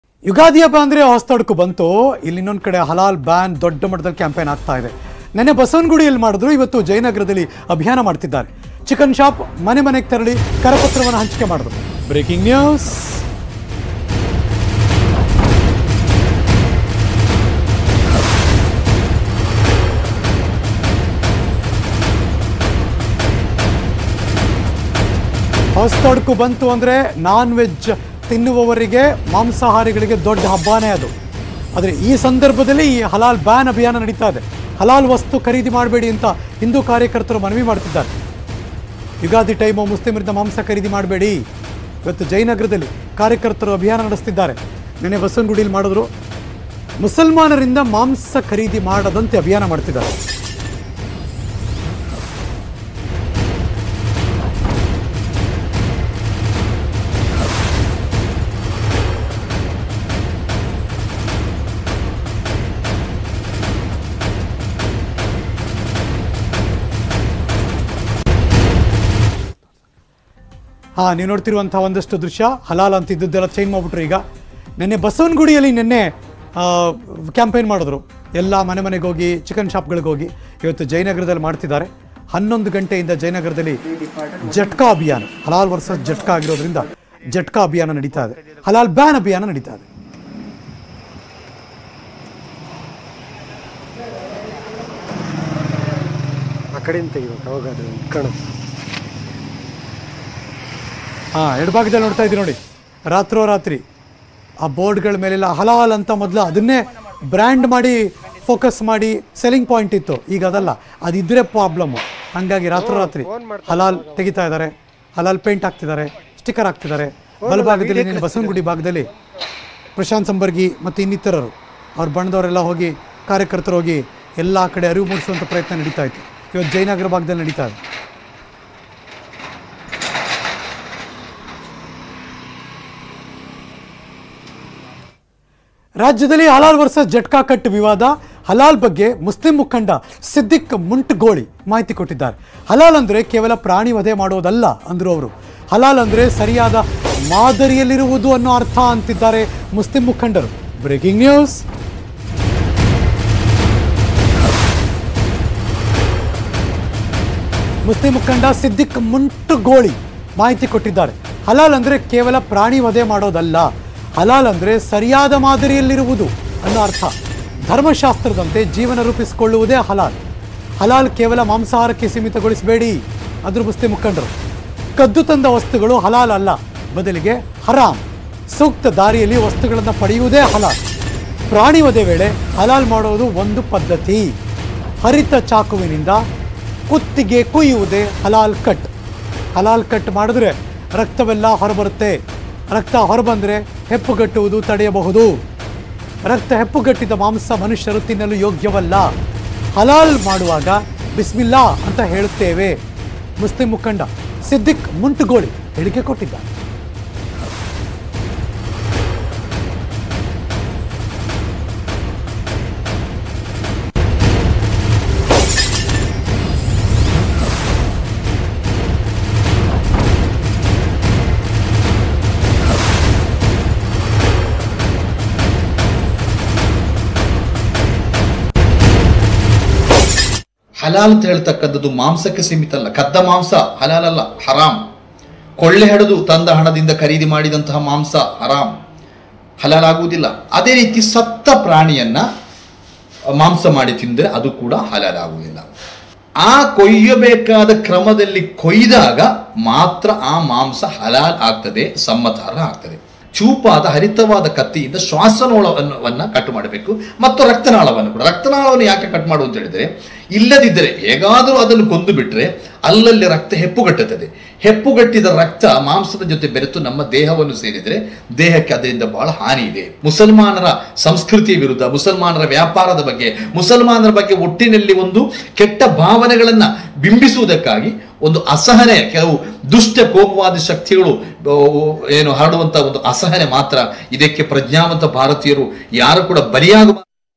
Jayanagarನಲ್ಲಿ Halal Boycott ಅಭಿಯಾನ; ಯುಗಾದಿ ವೇಳೆ ಮುಸ್ಲಿಂರಿಂದ ಮಾಂಸ ಖರೀದಿ ಮಾಡದಂತೆ ಮನವಿ
Halal Boycott | ಇಂದು Jayanagarನಲ್ಲಿ ಹಲಾಲ್ ಬಾಯ್ಕಾಟ್ ಅಭಿಯಾನ ಶುರು ಮಾಡಲಾಗುತ್ತಿದೆ. ಮುಸಲ್ಮಾನರಿಂದ ಮಾಂಸ ಖರೀದಿಸದಂತೆ ಕರೆ ನೀಡಲಾಗಿದೆ.